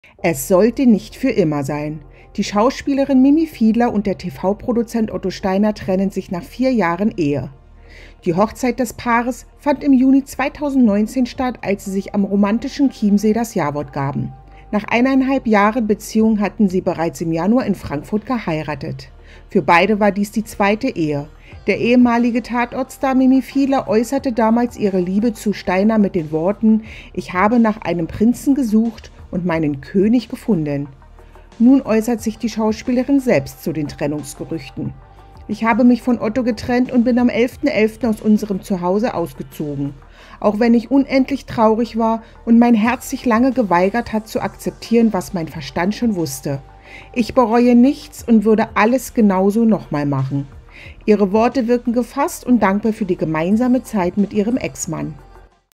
Mimi Fiedler - Nach Ehe-Aus: "Ich bereue nichts"
Schauspielerin Mimi Fiedler hat sich von Otto Steiner getrennt. Auf Instagram gibt sie jetzt Einblicke in ihr Seelenleben nach der Trennung.

Impressum:
Burda Forward
St.-Martin-Straße 66
81541 München